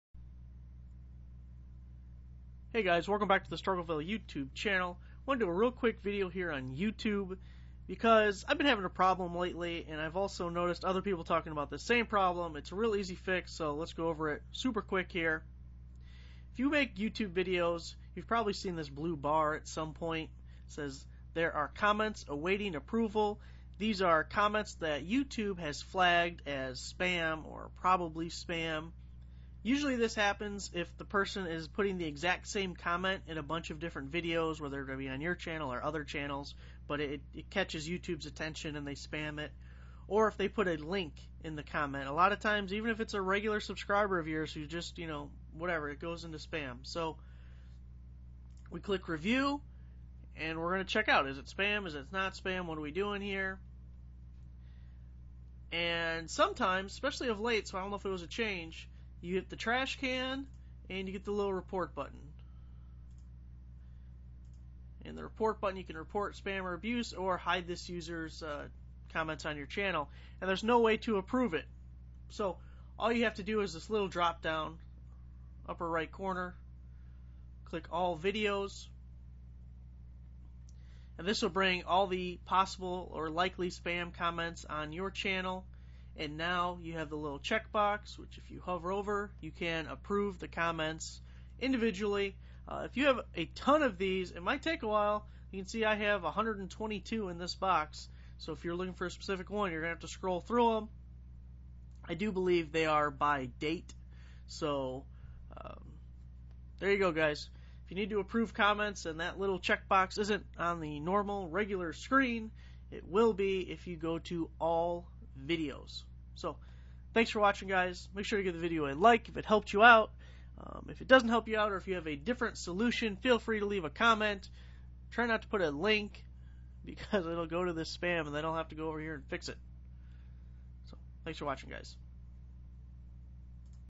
'Can't Approve Youtube Comments' EASY FIX (Updated For 2020)
If you are having a problem with Youtube sending non-spam comments into your spam folder and you are not able to approve these comments, this is your fix!